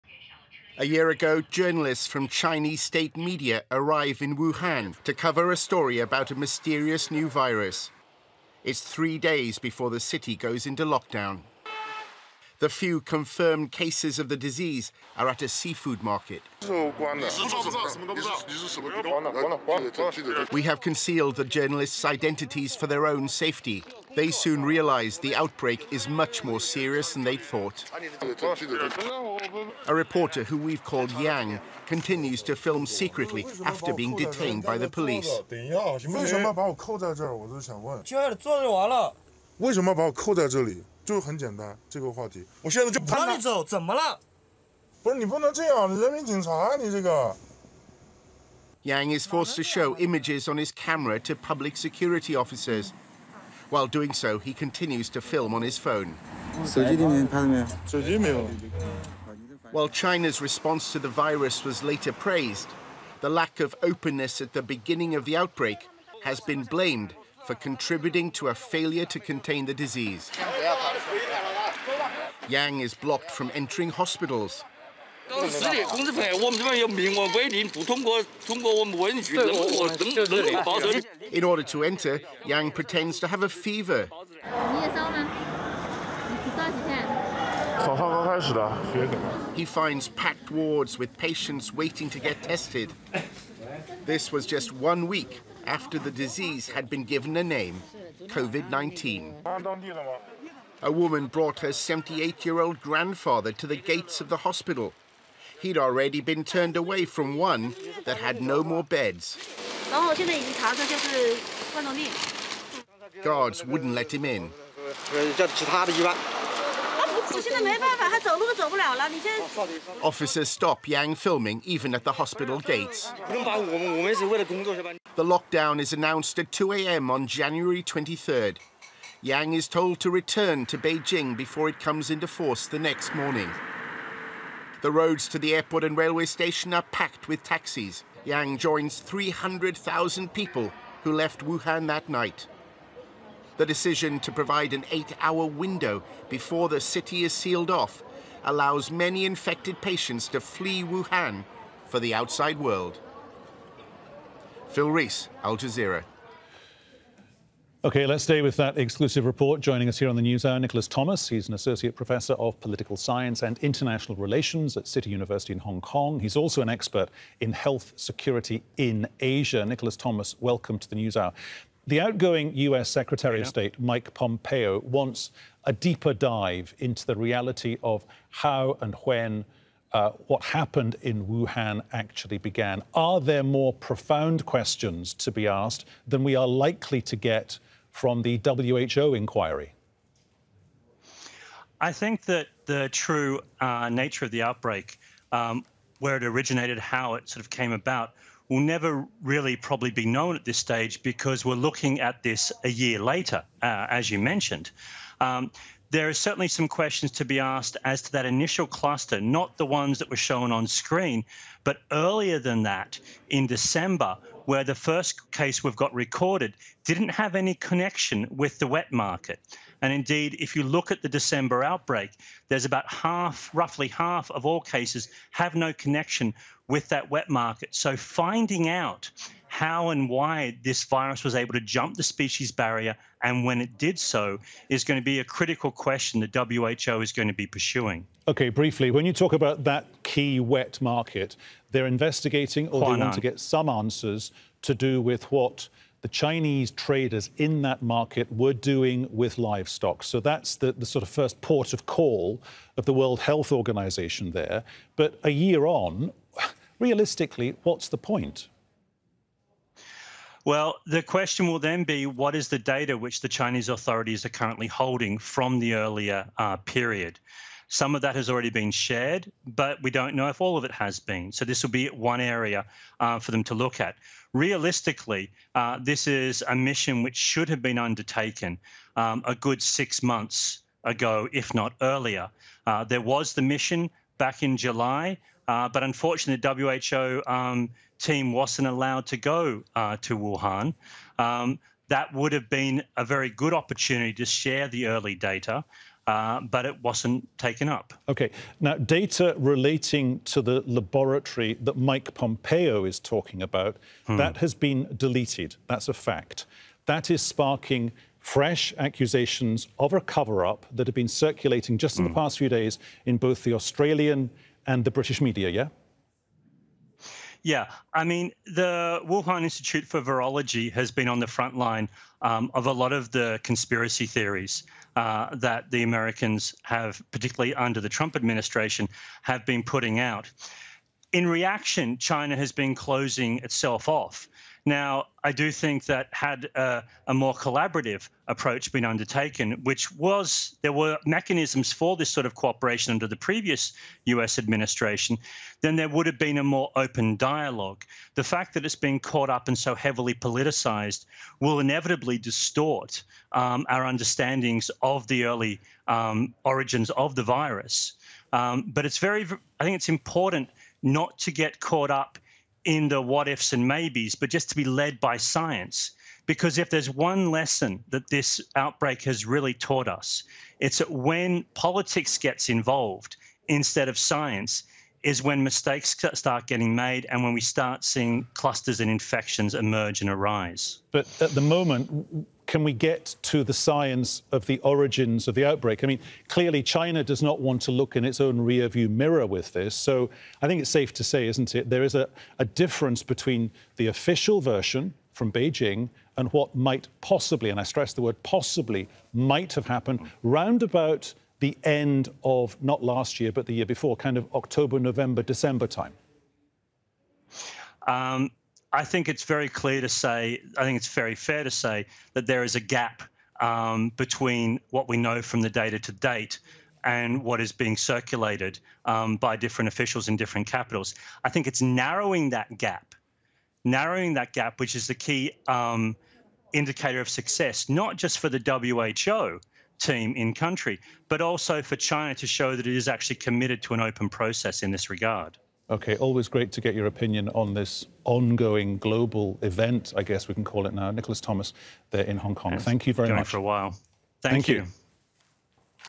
China: Videos expose earliest days of Wuhan's COVID lockdown
It was this week, one year ago, that the world started becoming aware of the seriousness of a viral outbreak which would explode into a full-blown global pandemic.

Wuhan, in China, was the first place to go into lockdown.

Al Jazeera's Investigative Unit has obtained videos recorded during the earliest days - which were smuggled out of the country.

Al Jazeera's Phil Rees has this exclusive report.